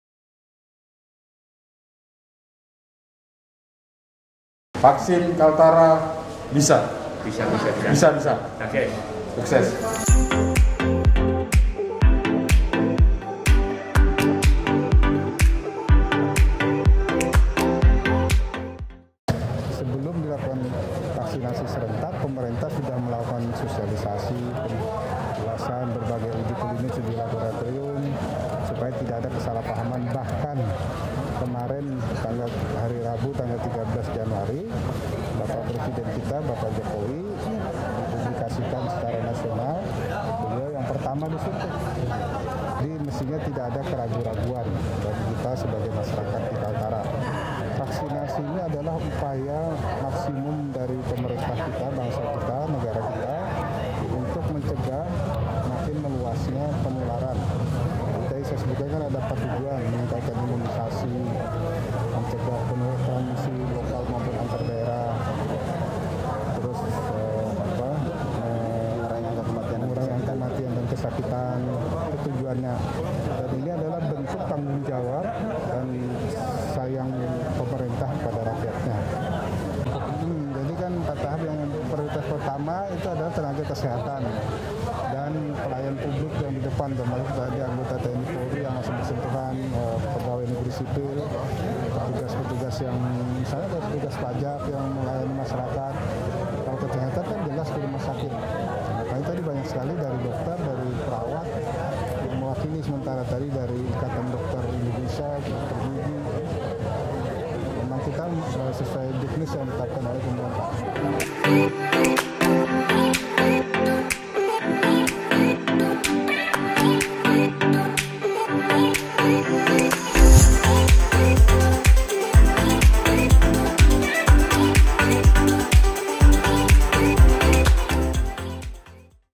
Pelaksanaan Vaksinasi Covid-19 di Kaltara
Vaksinasi digelar serentak se-Indonesia pada hari ini (14/1/2021). Di Kaltara, vaksinasi yang pada tahap 1 menyasar tenaga kesehatan ini dimulai pagi tadi di dua daerah, Kota Tarakan dan Kabupaten Bulungan. Gubernur Kalimantan Utara (Kaltara) Dr H Irianto Lambrie menyaksikan langsung vaksinasi serentak tersebut di Puskesmas Tanjung Selor.